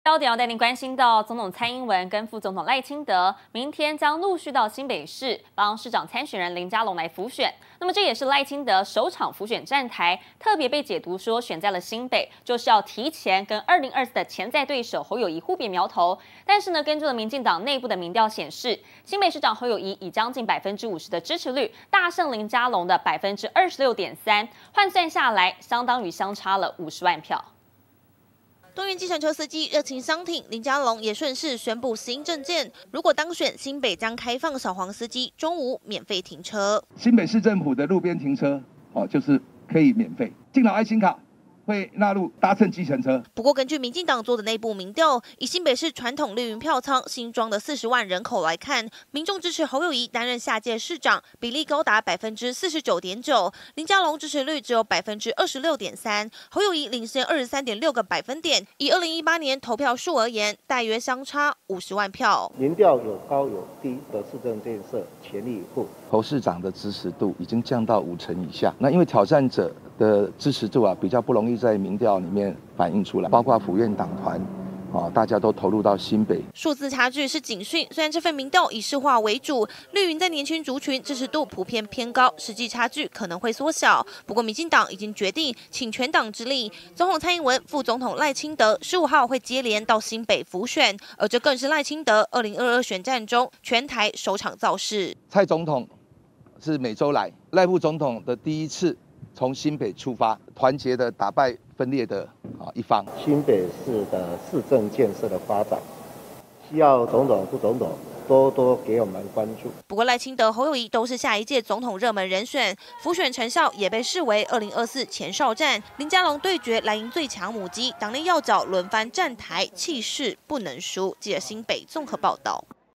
全黨挺林佳龍! 正副總統15日接力站台輔選
總統蔡英文和副總統賴清德，明天(15號)將陸續到新北市，幫黨提名的市長參選人林佳龍輔選，這也是賴清德首場輔選站台，特別選在新北，也被解讀是要提前和2024的潛在對手侯友宜互別苗頭。不過根據民進黨內部在新北傳統綠營票倉做的民調，新北市長侯友宜以近50%的支持率大勝林佳龍23.6%個百分點，換算下來相當於相差50萬票。